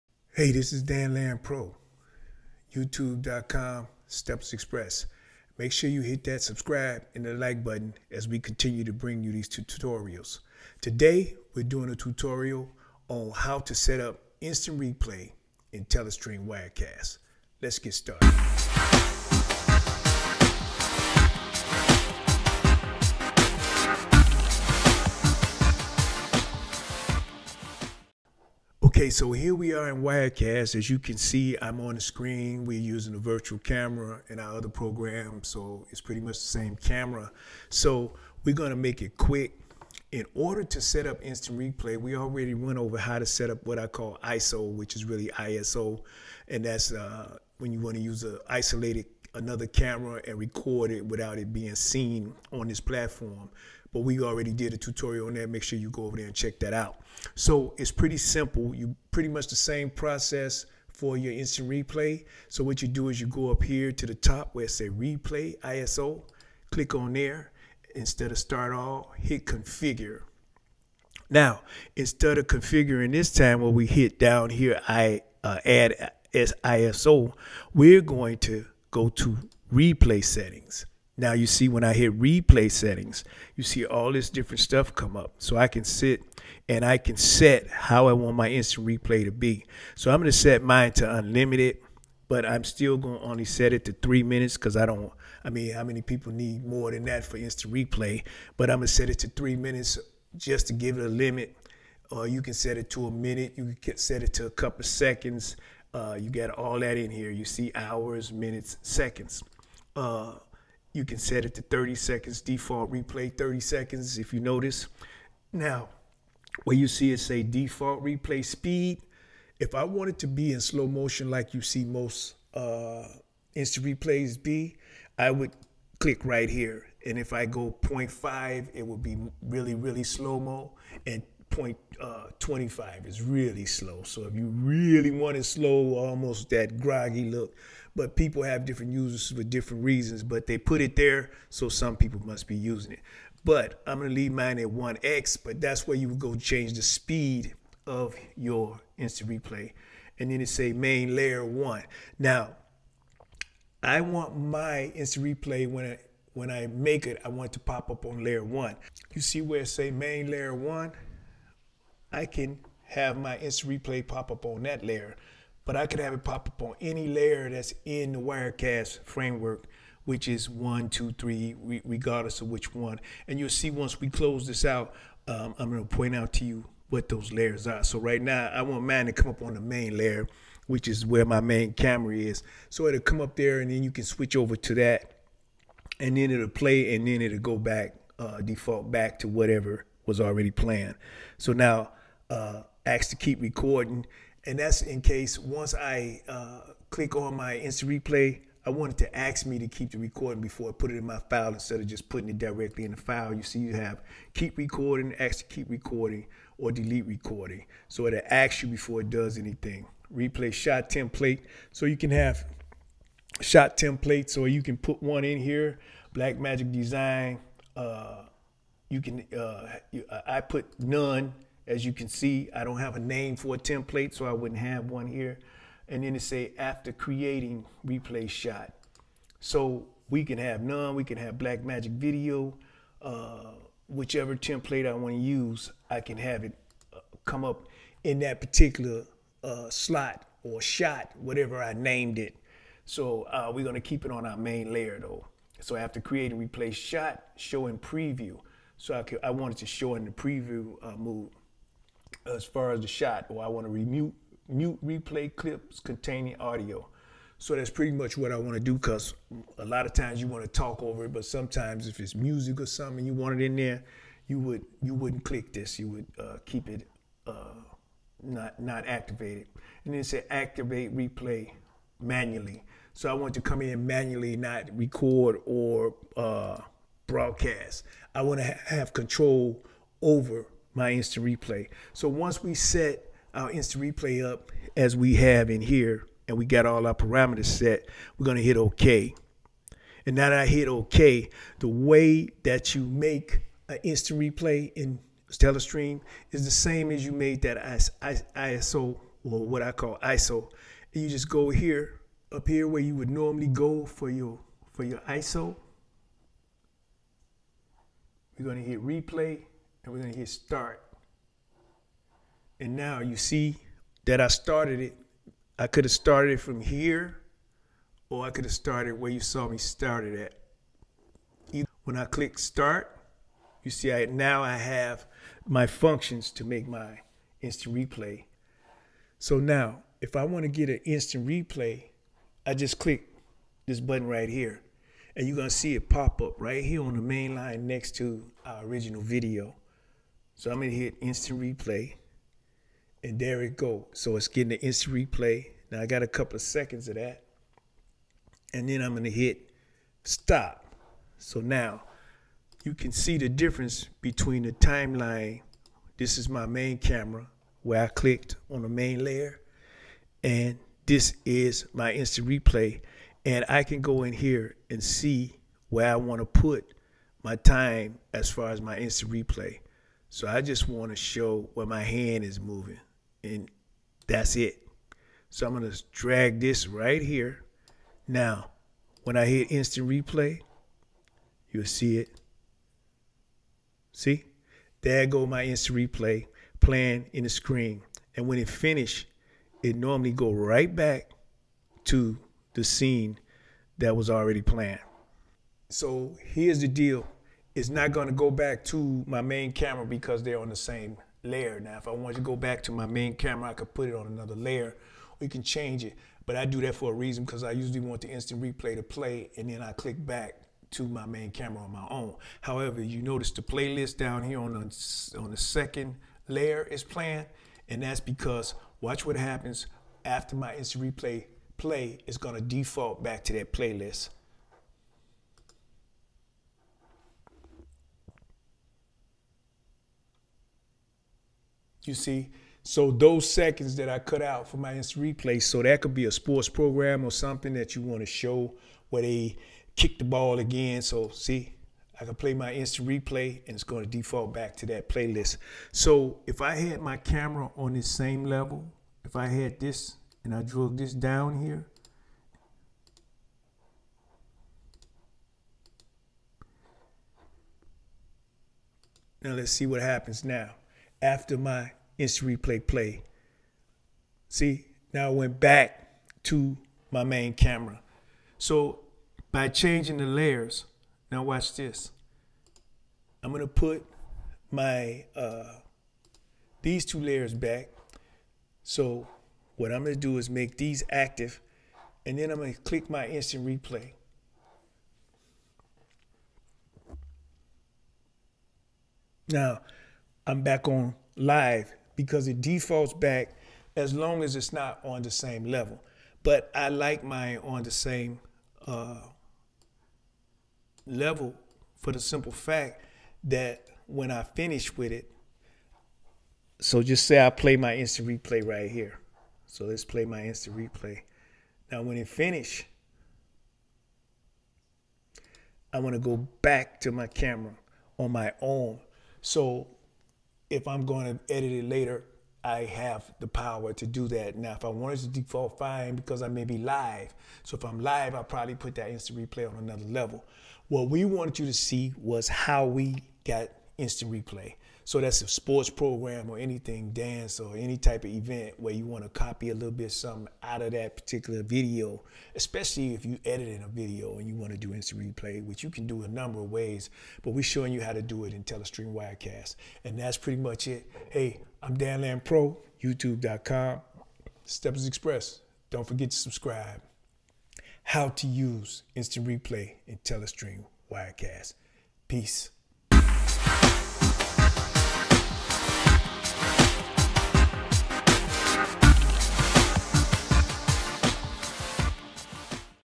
How to configure Instant Replay in Telestream Wirecast Pro
A  overview about how I configure Instant Replay for action events in TeleStream Wirecast.